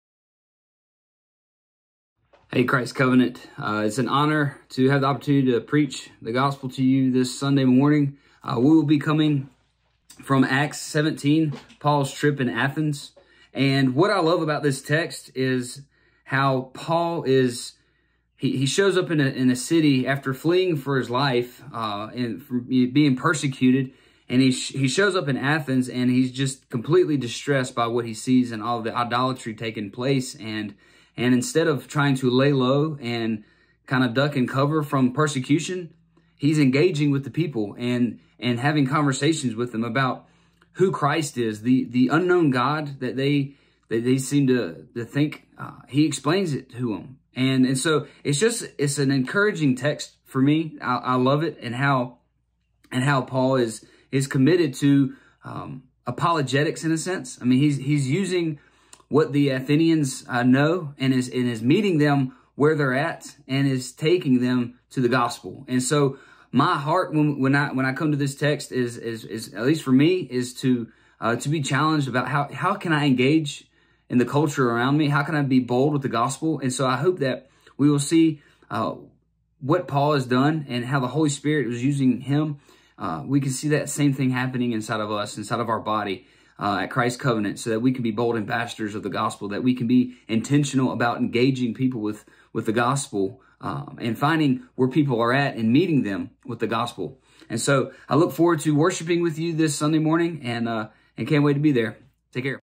Sermon Preview for 6.13
Join us Sunday as we continue our series on selected texts in Acts. We will be in Acts 17 this Sunday!